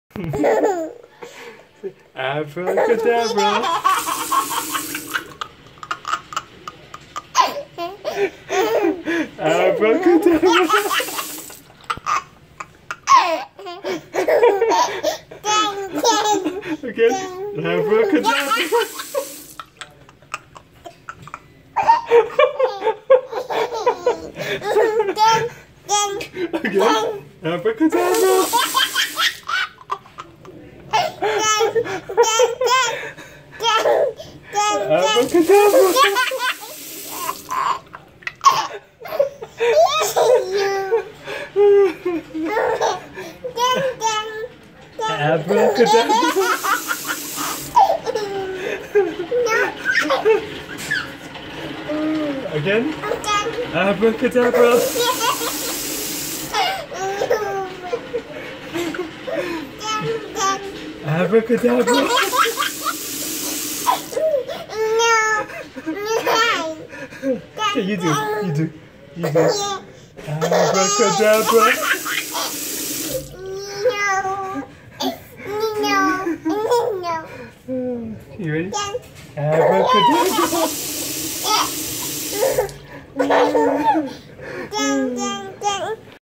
Alinea's baby laughs with the magic water faucet! You will laugh so hard your tummy will hurt!
Alinea thinks the automatic water faucet is hilarious!